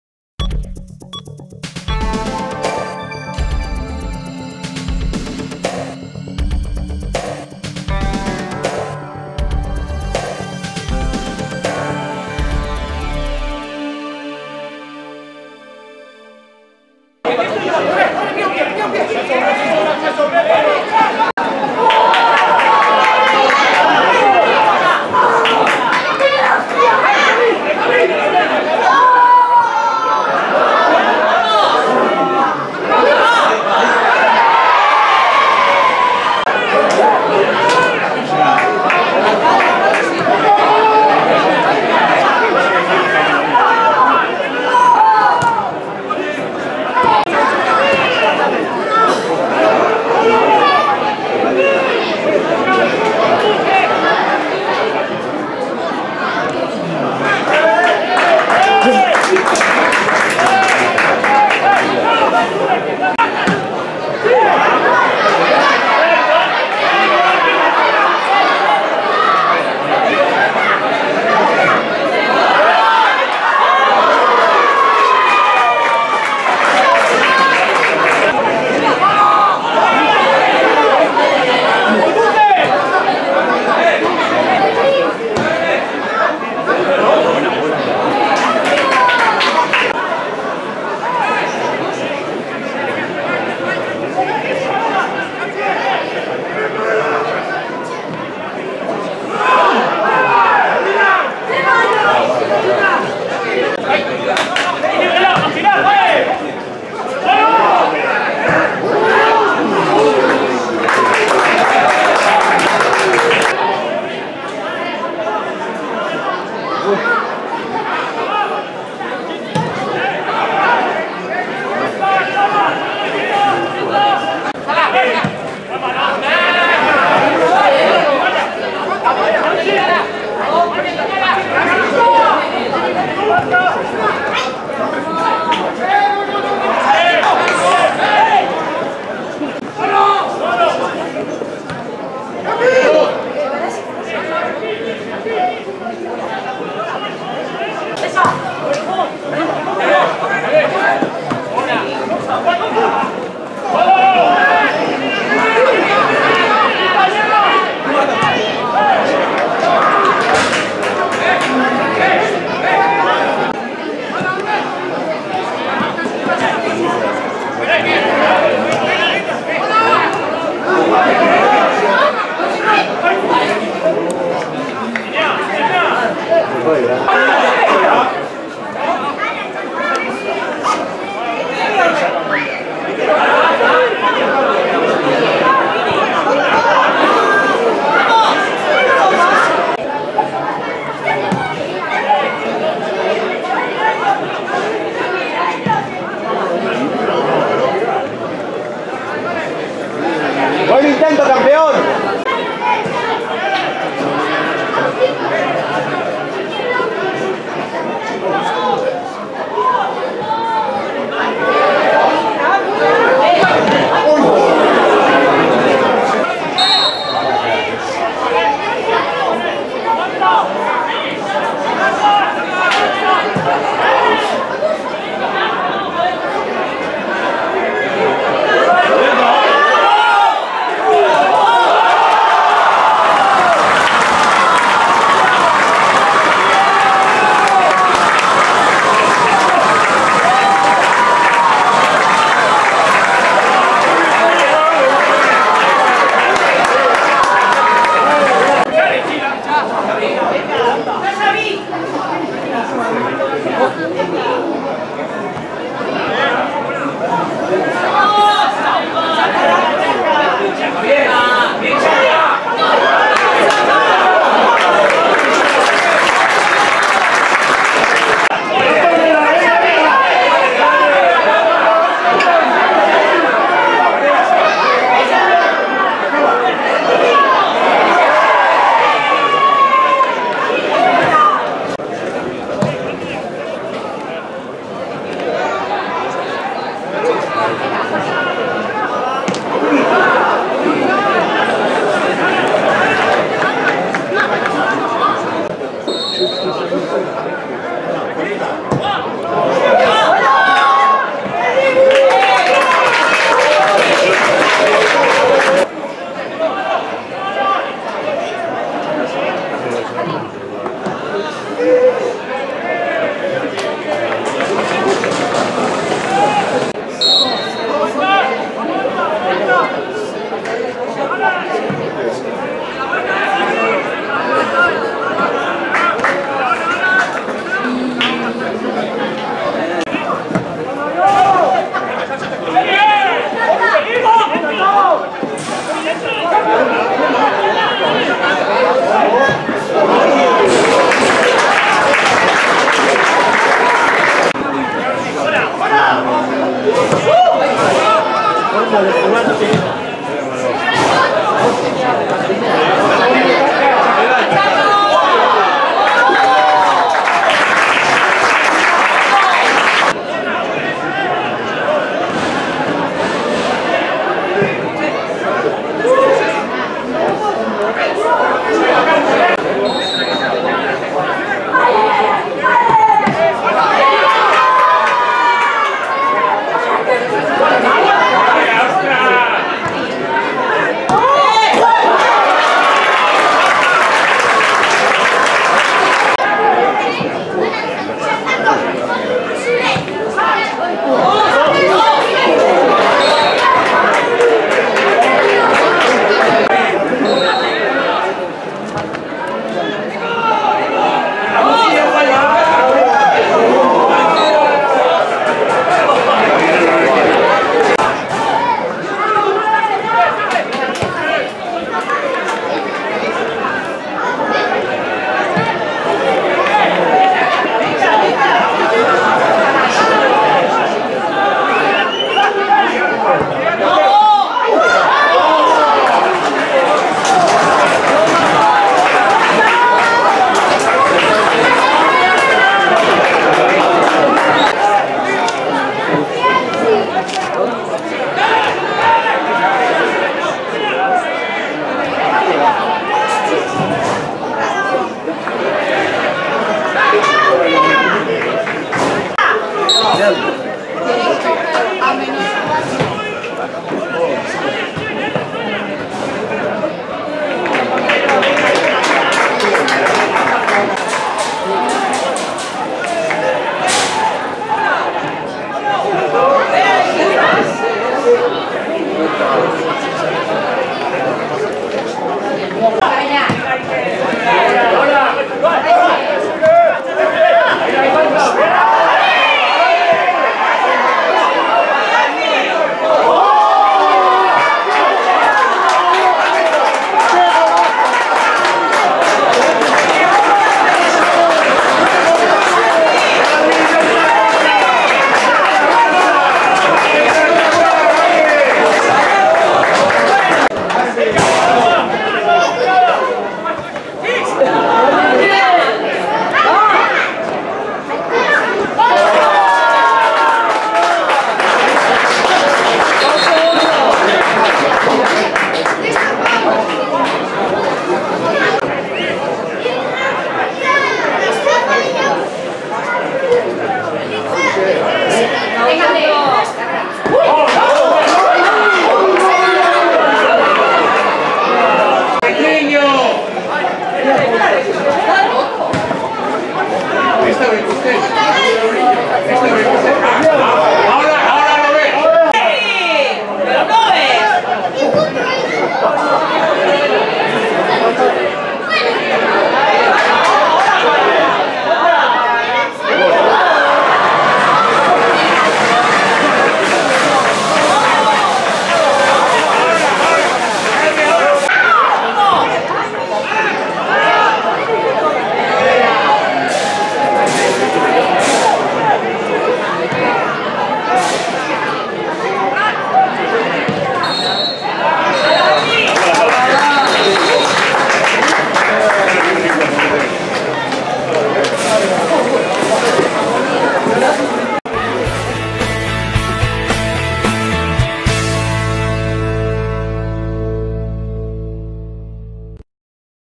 AURRERA K.E. 2:3 Beasain K.E. (Zaldupe, 2024/02/18)
2024ko Otsailaren 18an Ondarroako Zaldupe futbol zelaian jokatu zen partiduaren laburpena.